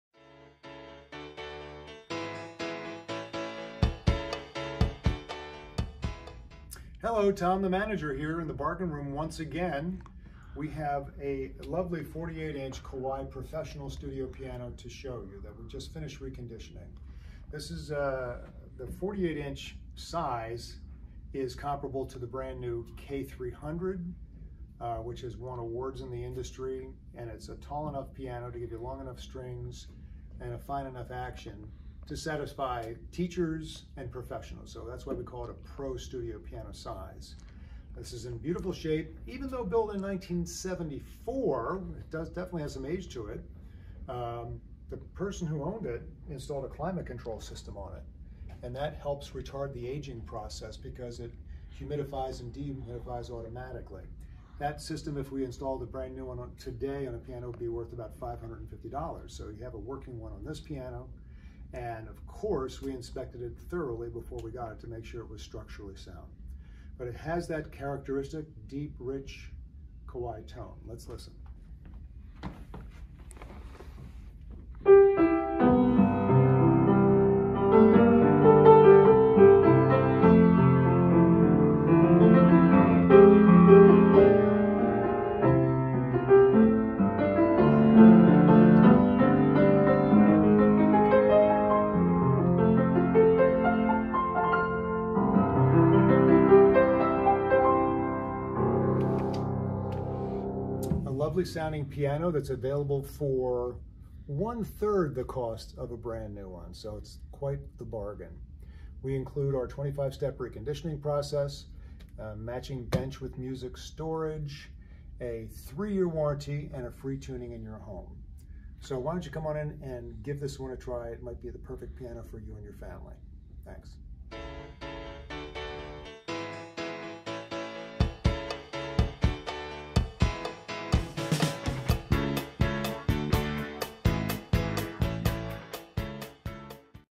Used Kawai 48" Model BL-12 Professional Studio Piano
Kawai 48” professional studio piano: Kawai is a Japanese precision engineered piano company that has won many awards in the industry. This predecessor to the current Kawai K300 studio offers professional quality at one third the cost of a new model! Though built in 1974, this fully reconditioned piano has many years of fine performance left in it, with responsive touch and a full, rich tone. It also includes a Climate Control System! Also included is our 25 step reconditioning process, matching bench and 3 year warranty. We also include one free tuning in the home.